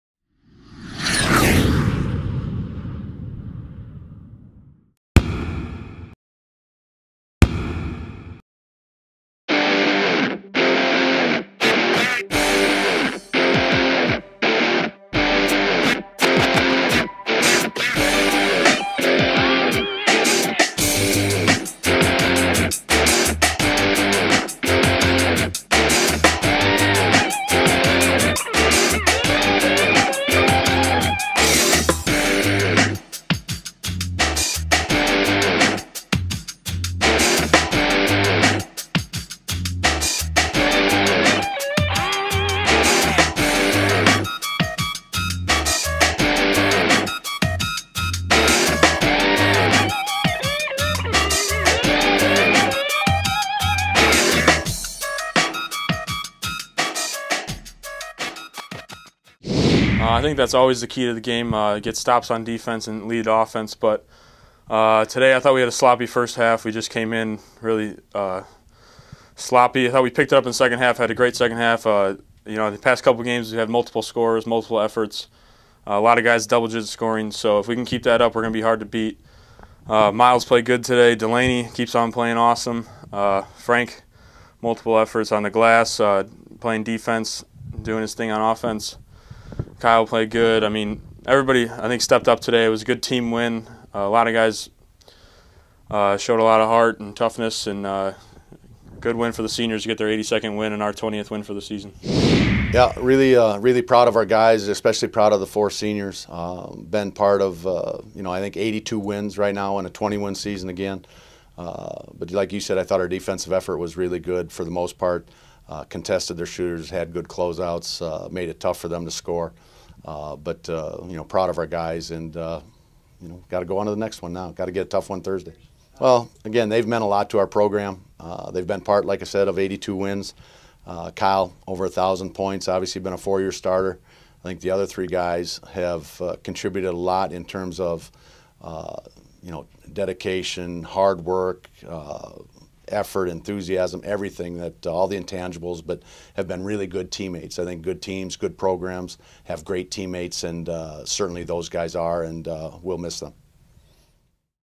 Lewis Men's Basketball vs Saint Joseph's (W 87-62) 2-20-16
Highlights and interviews from Lewis Men's Basketball's 87-62 win against Saint Joseph's 2-20-16